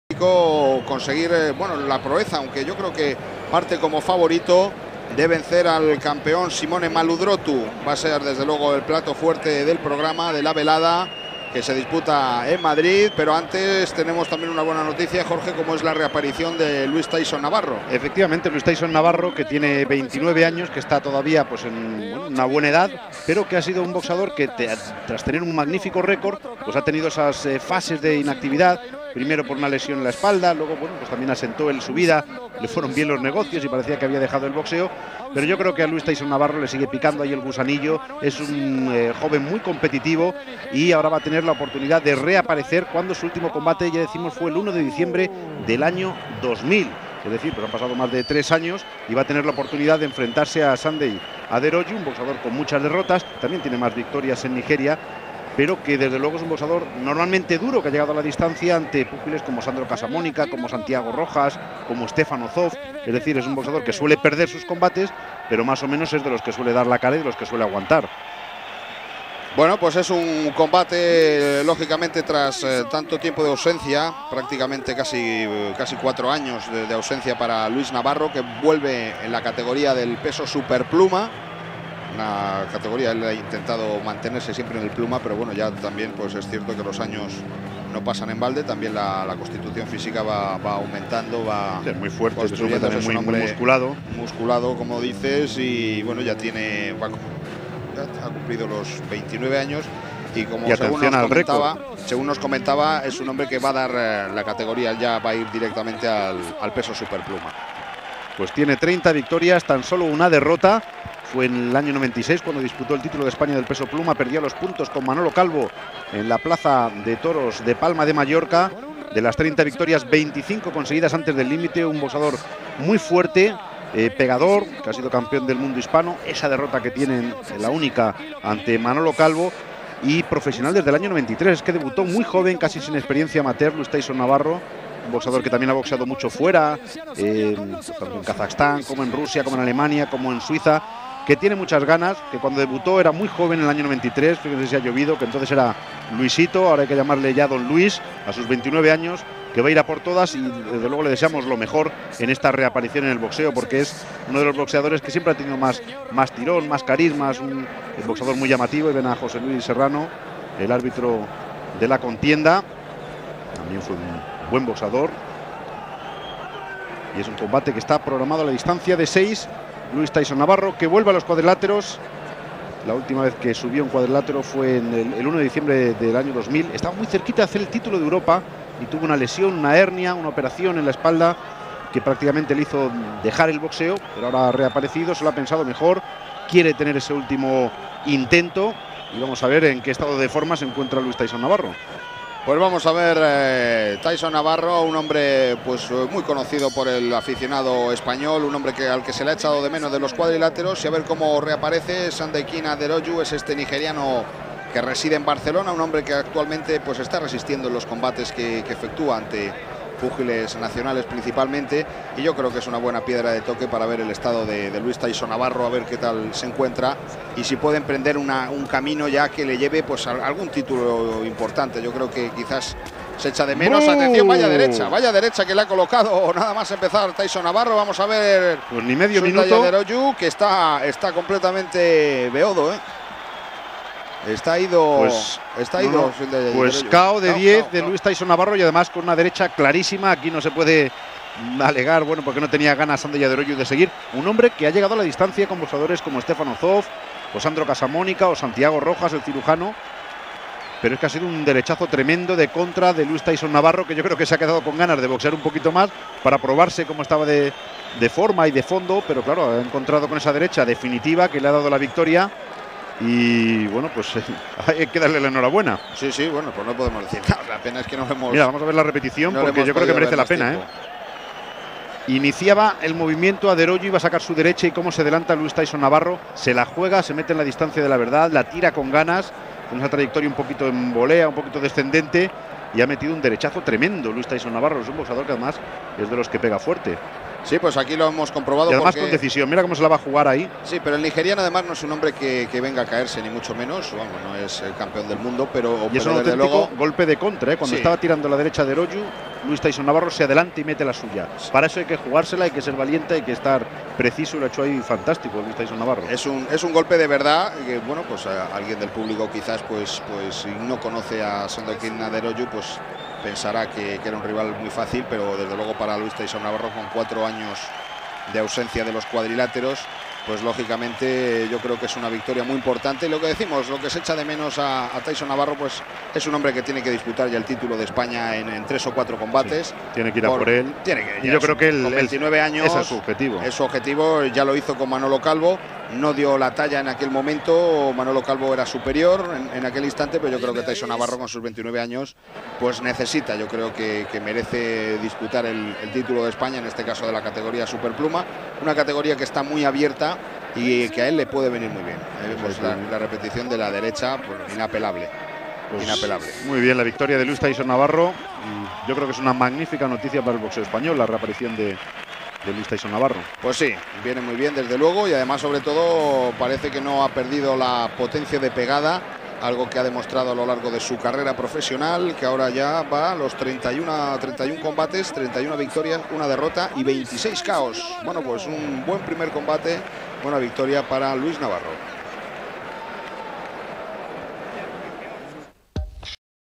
Luis "TYSON" NAVARRO 🇪🇸 vs 🇳🇬 Sunday ADEROJU [29-10-2004]
Último combate como profesional del madrileño Luis "Tyson" Navarro.